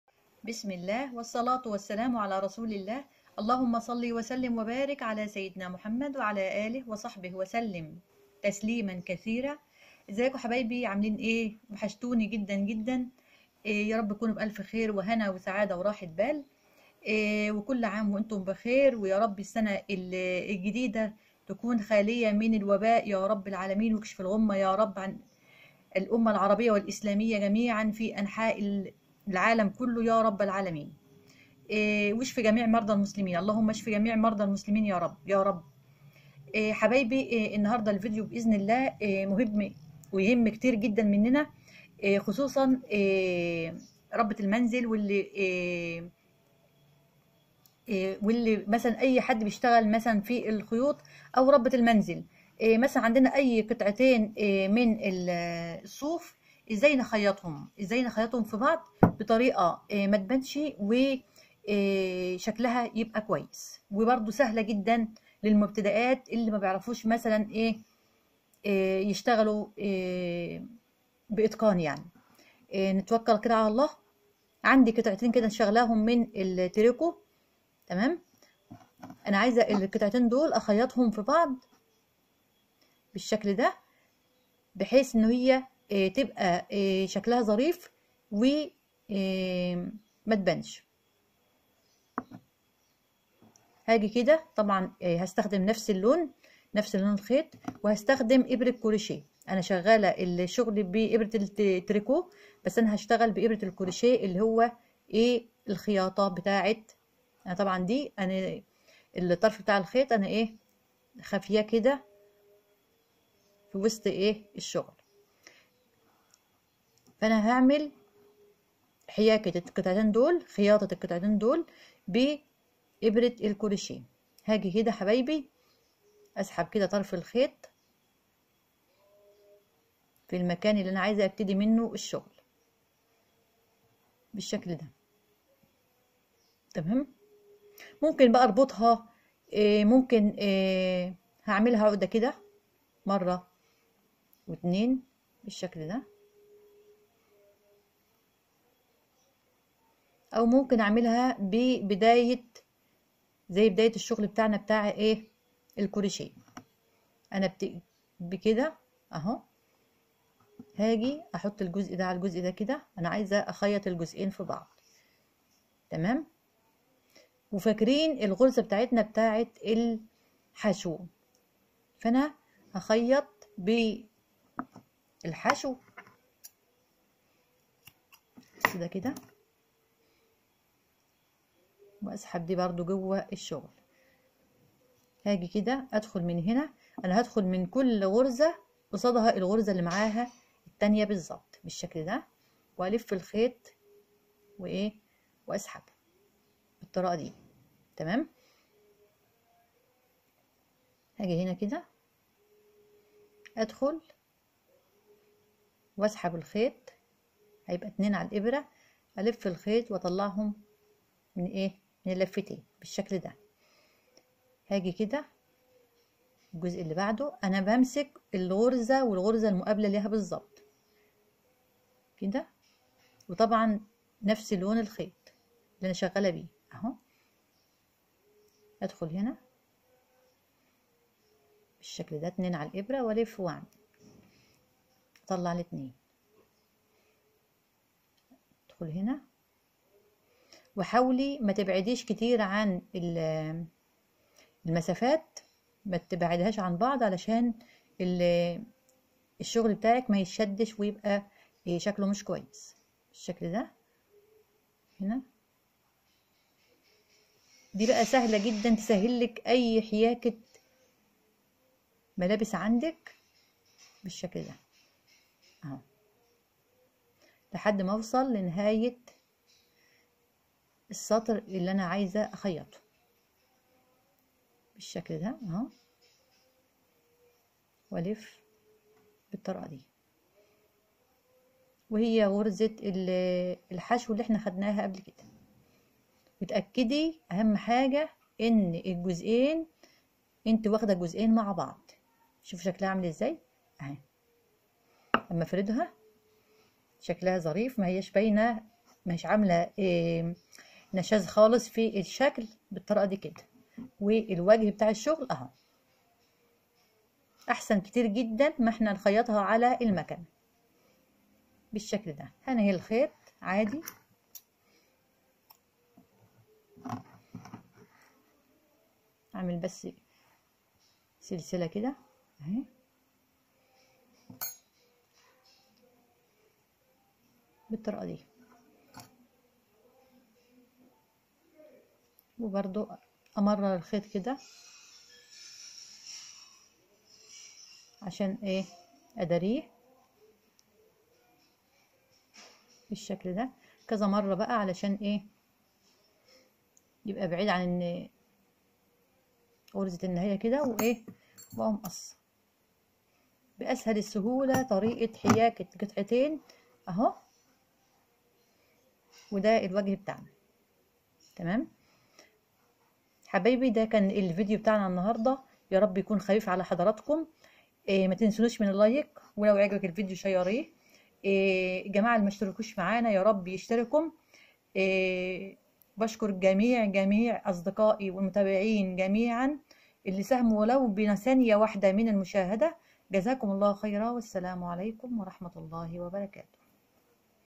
إزاي اخيط قطعتين صوف بغرزة واحدة
ازاي# اخيط#الصوف،ازاي اخيط قطعتين صوف بغرزة واحدة، في الفيديو ده نتعلم كيفية خياطة قطعتين من الصوف باستخدام إبرة الكروشيه وغرزة الحشو فقط بغرزة واحدة تقدري تخيطي أي ملابس صوفية ،حياكة الملابس الصوفية،خياطة أشغال الصوف بأسهل الطرق